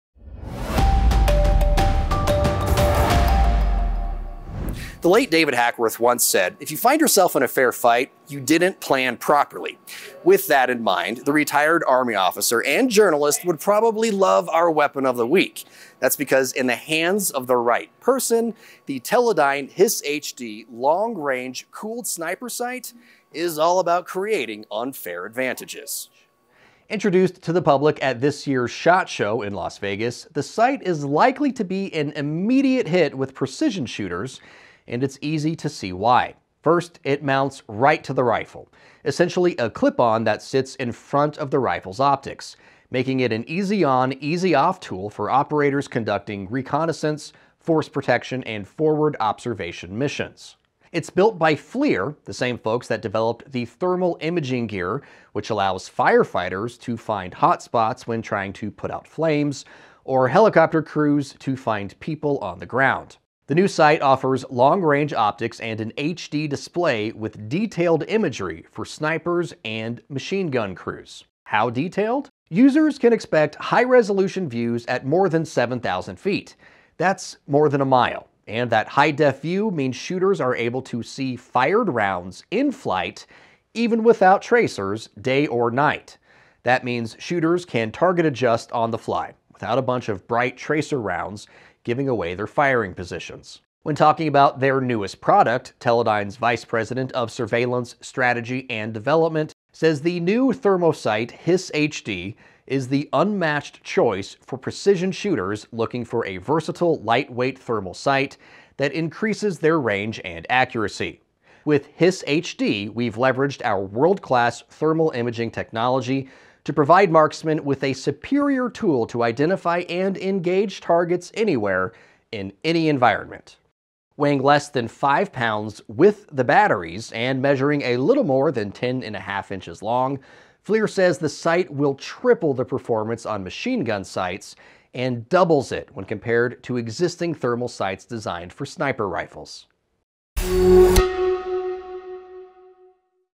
HISS-HD sight an ‘unmatched choice’ for precision shooters: Weapon of the week
Introduced to the public at the 2024 SHOT show in Las Vegas, Nevada, the Teledyne HISS-HD long-range cooled sniper sight is all about giving the user an unfair advantage. This sight is likely to be an immediate hit — especially for precision shooters.